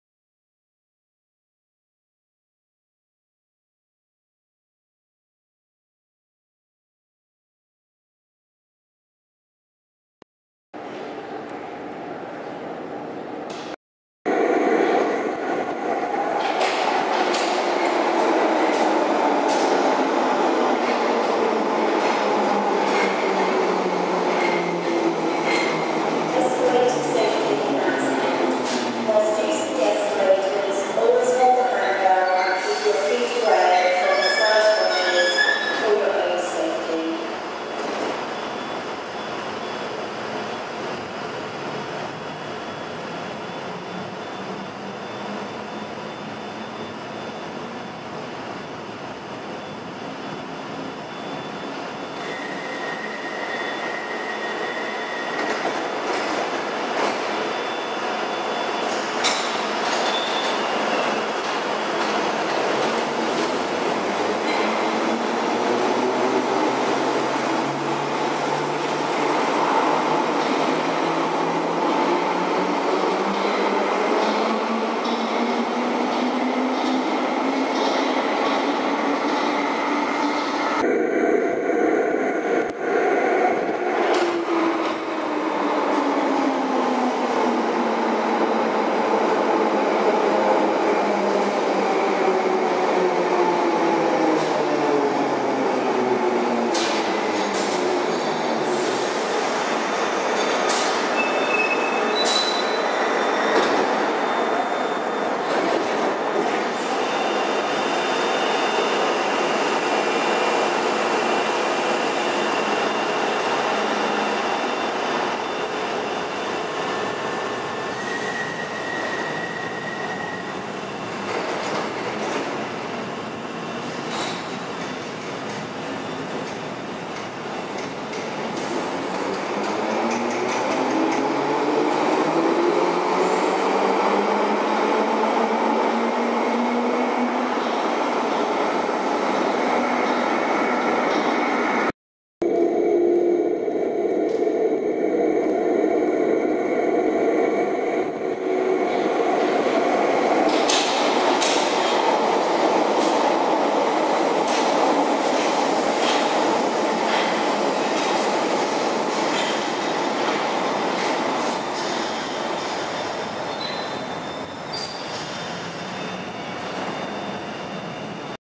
London Underground Bakerloo Line 1972 Stock Trains At Marylebone 21 May 2019
Some southbound London Underground Bakerloo Line 1972 Stock trains seen at Marylebone during the early evening of Tuesday 21 May 2019.